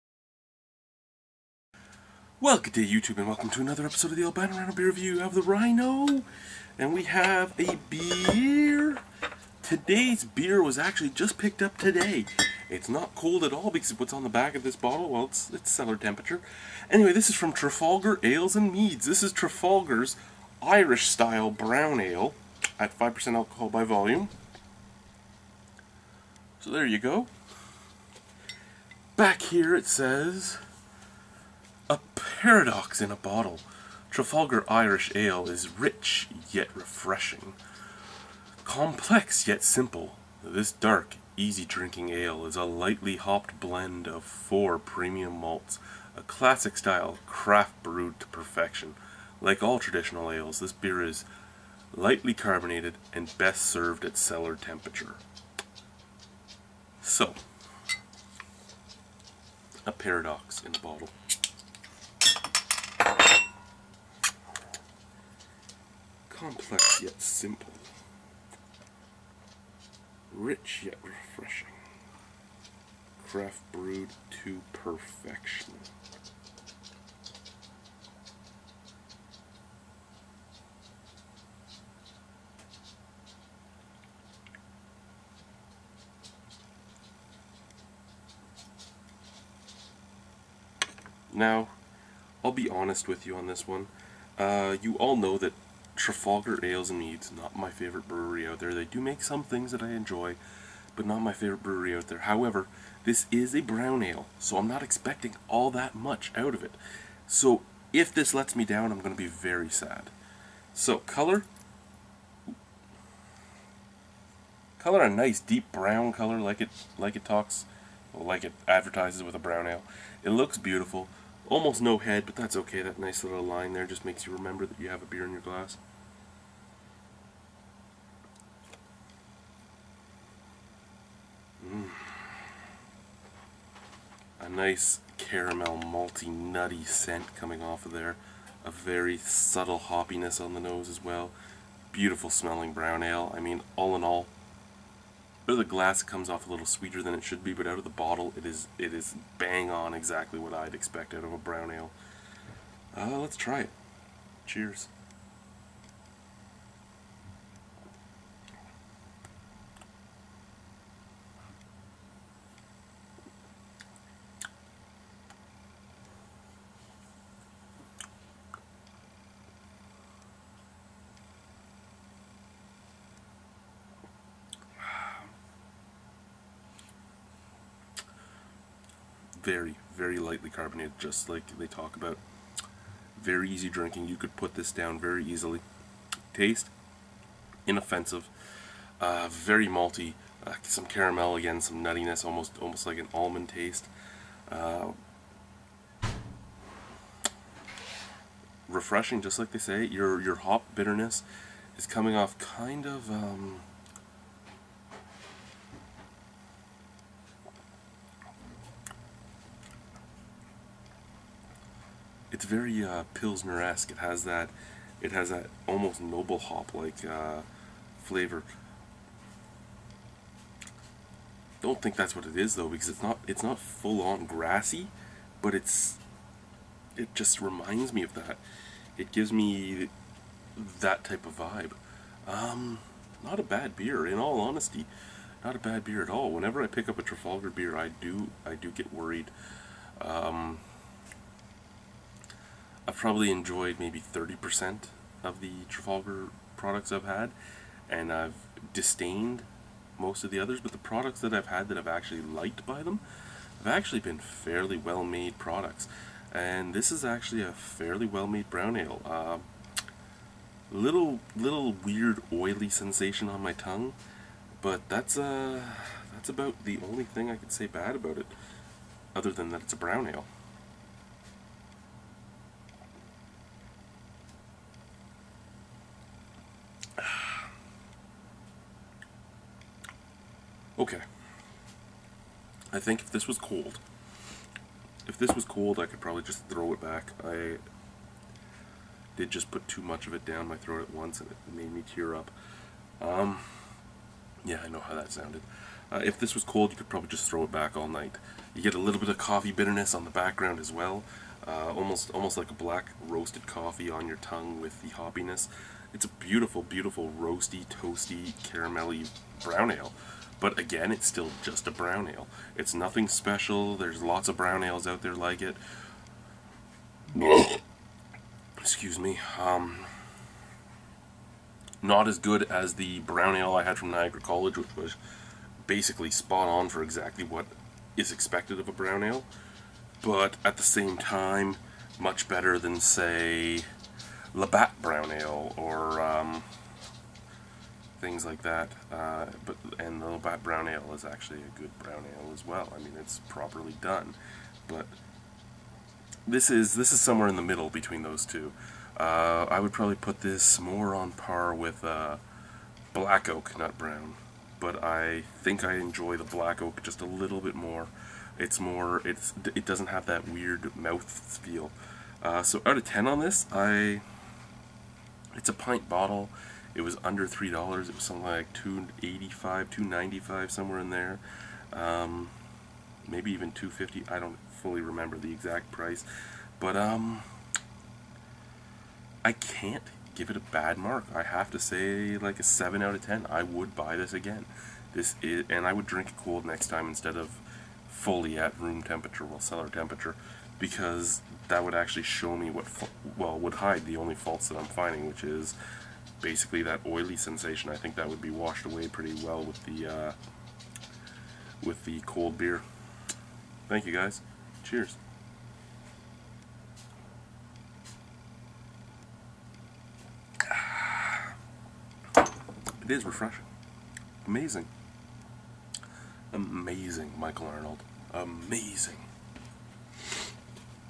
Trafalgar Irish Style Brown Ale : Albino Rhino Beer Review
In this beer I think Irish style just denotes it's alcoholic.